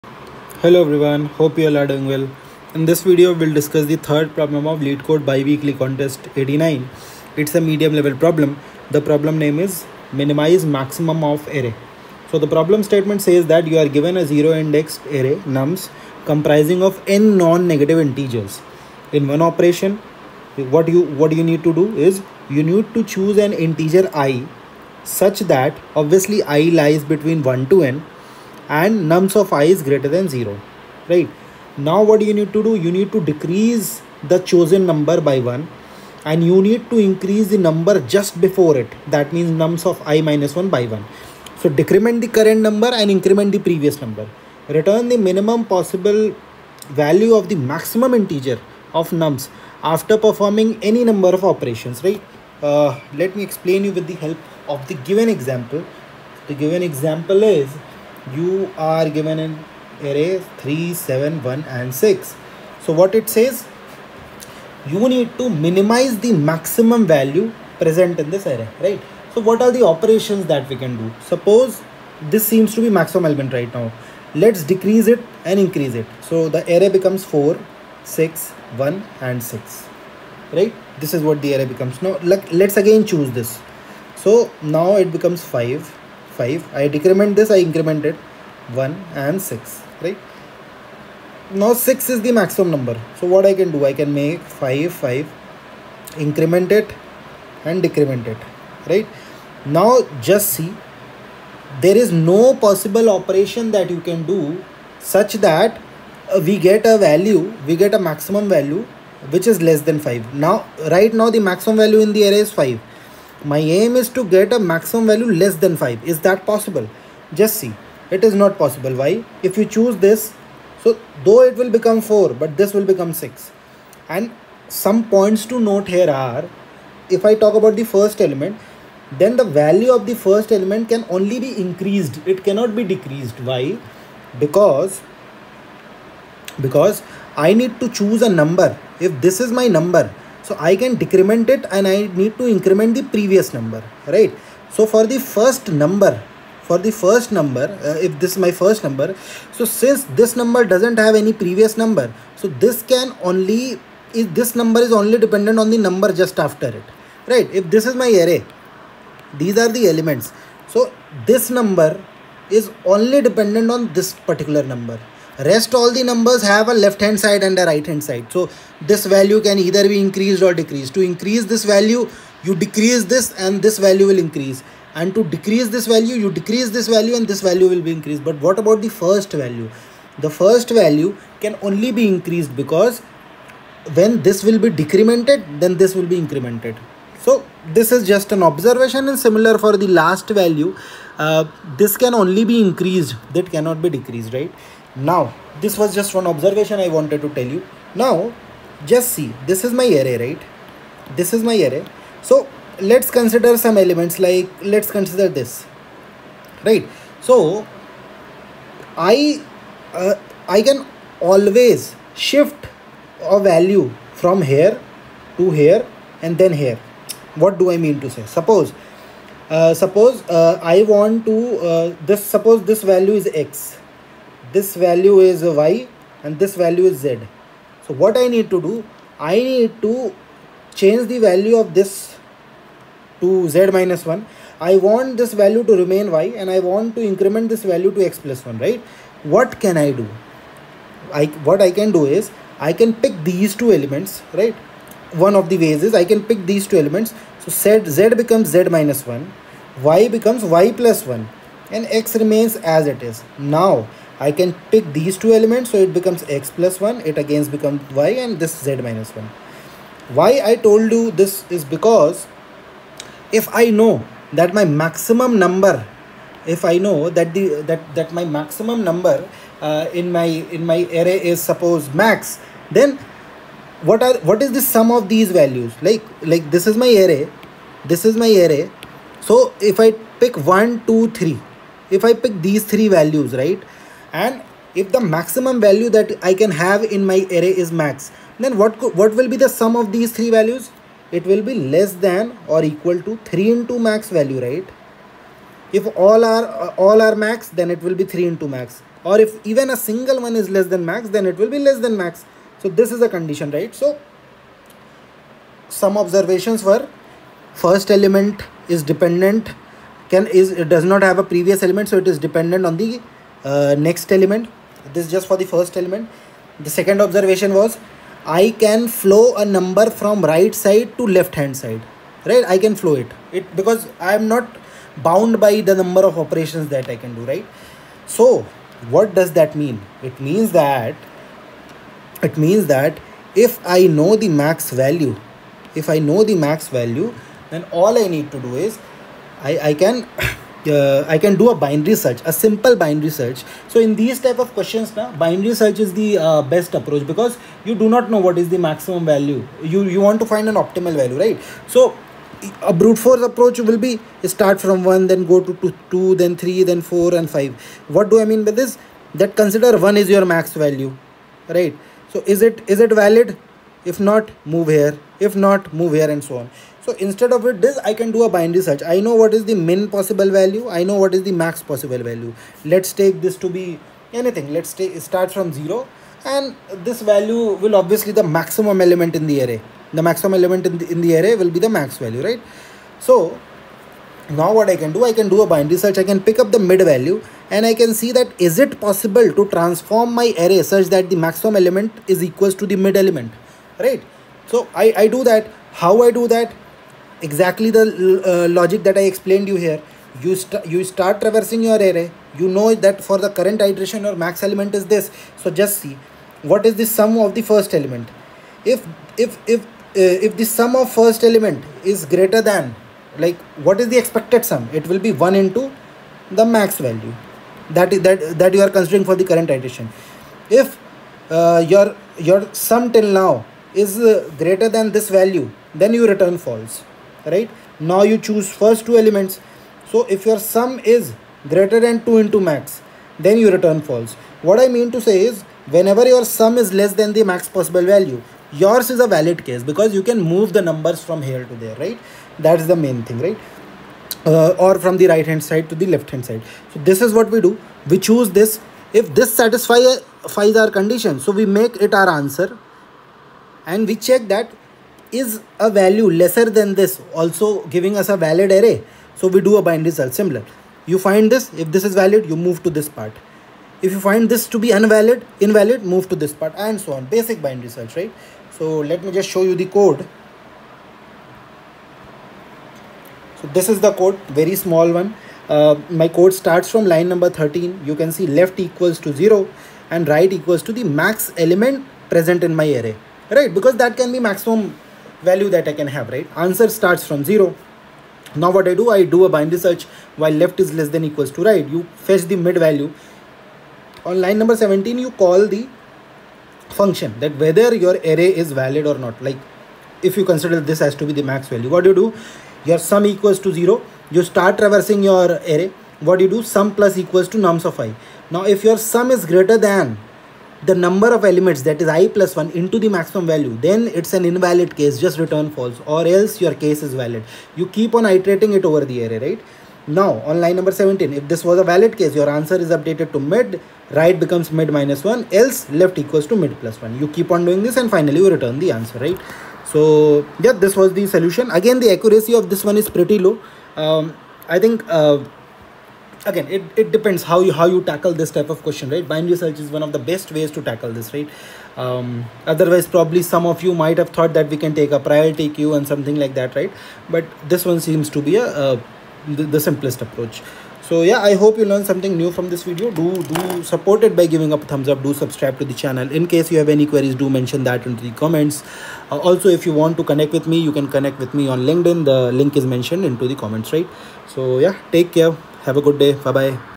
Leetcode BiWeekly contest 89 - Medium - Minimize Maximum of Array
In this video we discuss the third problem of Leetcode BiWeekly contest 89